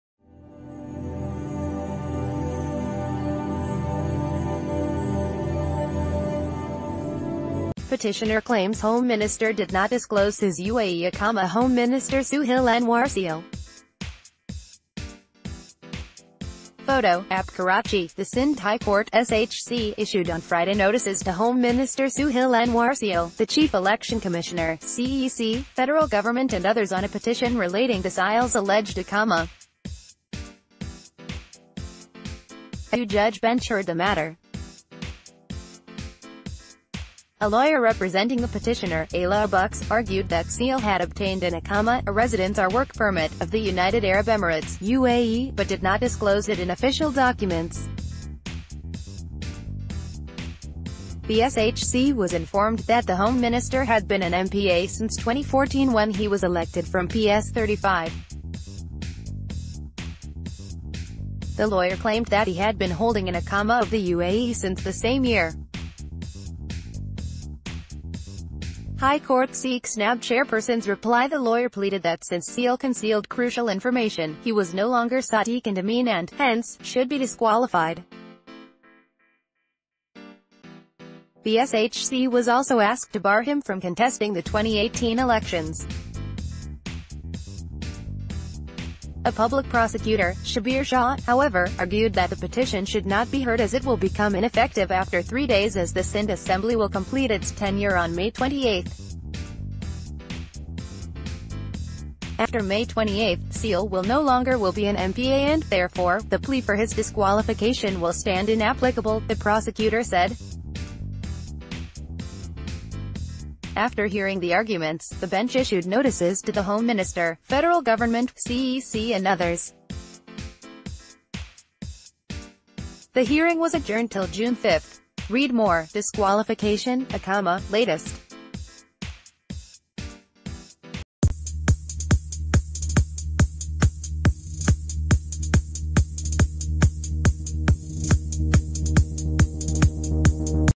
Breaking News | Disqualification case: SHC issues notices to Sohail Anwar Siyal, federal govt, chie
Breaking News | Disqualification case: SHC issues notices to Sohail Anwar Siyal, federal govt, chief election commissioner

Petiti­oner claims home minist­er did not disclo­se his UAE iqama Home Minister Sohail Anwar Siyal. PHOTO: APP KARACHI: The Sindh High Court (SHC) issued on Friday notices to Home Minister Sohail Anwar Siyal, the chief election commissioner (CEC), federal government and others on a petition relating to Siyal’s alleged iqama. A two-judge bench heard the matter. A lawyer representing the petitioner, Allah Bux, argued that Siyal had obtained an iqama – a residence or work permit – of the United Ara...